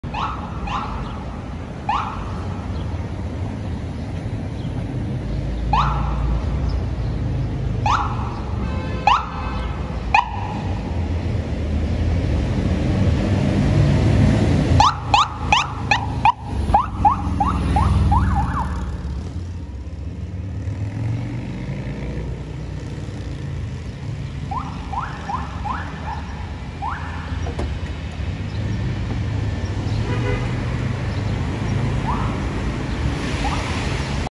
“RARE CATCH” OF 2 NYPD MOTORCYCLES RESPONDING ON AMSTERDAM AVENUE ON WEST SIDE OF MANHATTAN, NYC.
HERE YOU WILL SEE A “RARE CATCH” OF 2 NYPD MOTORCYCLES RESPONDING ON AMSTERDAM AVENUE ON THE UPPER WEST SIDE AREA OF MANHATTAN IN NEW YORK CITY.